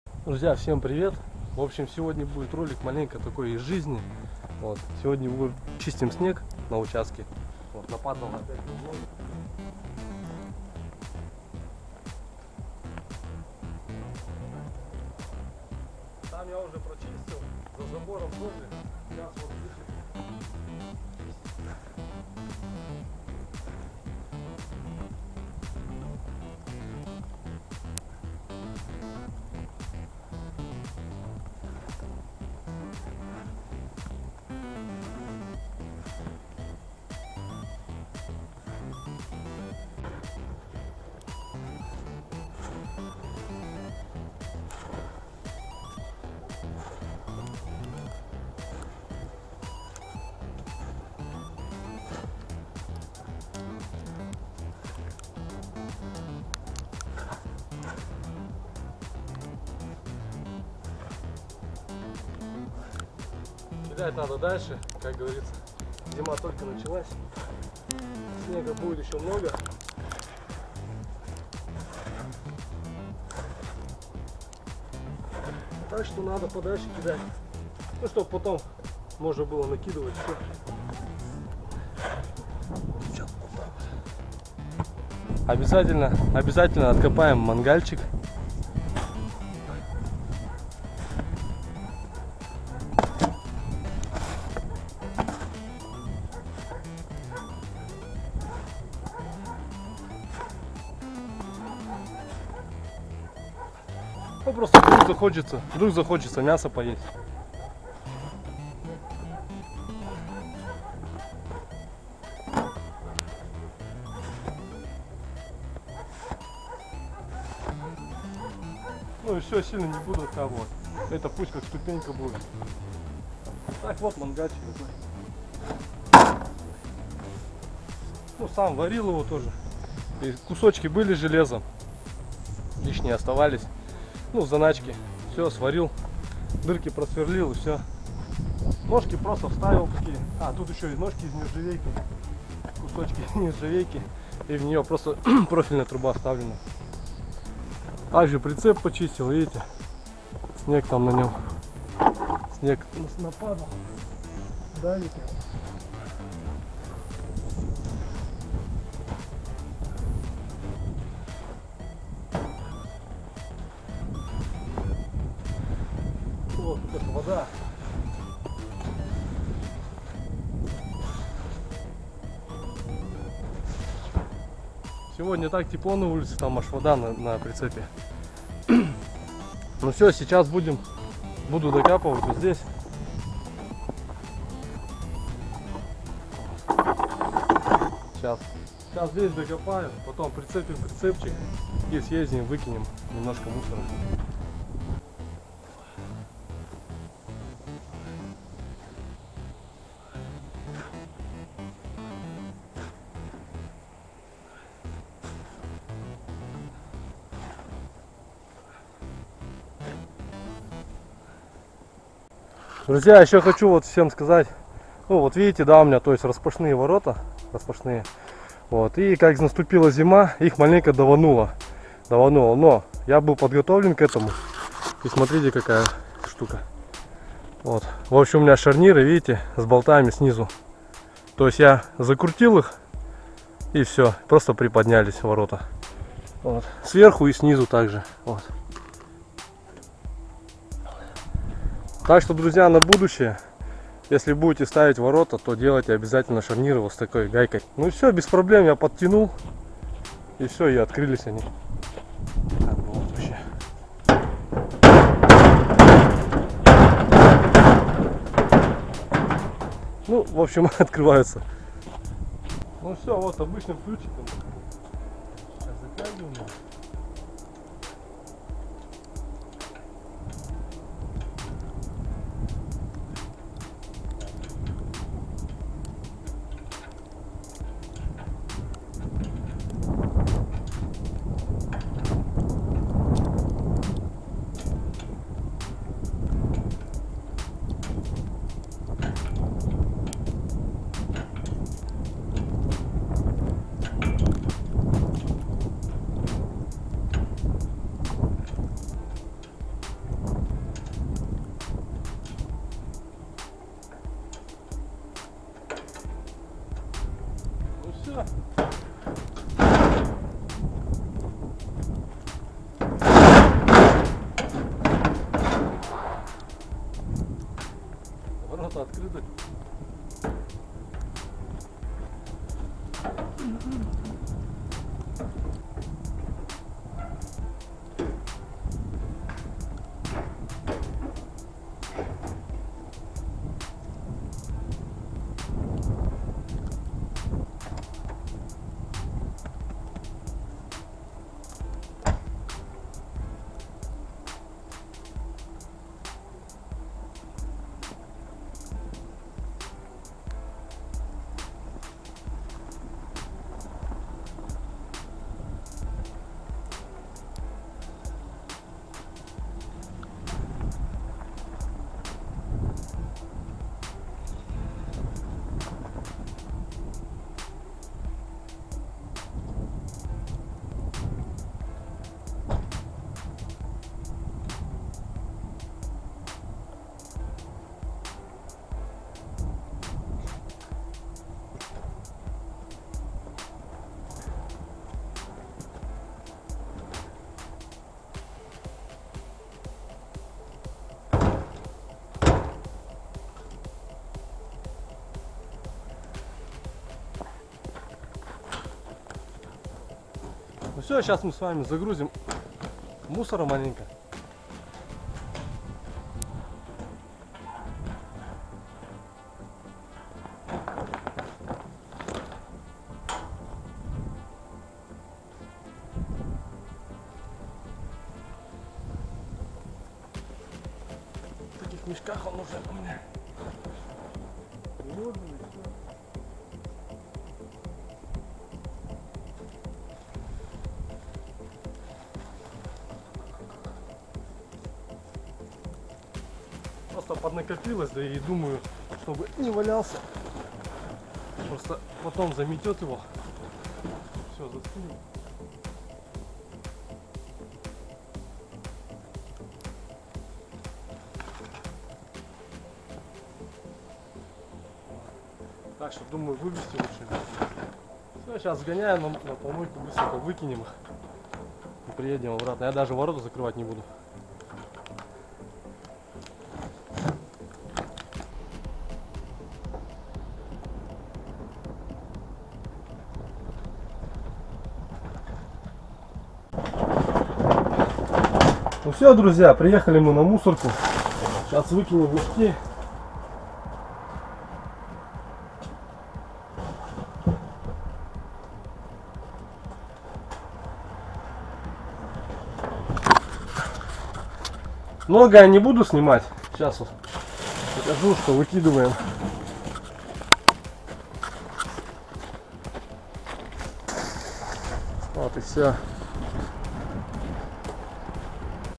Снег с распашными воротами и мусор.
Сегодня убираем снег на участке, показываю что сделал на распашные ворота и вывозим мусор.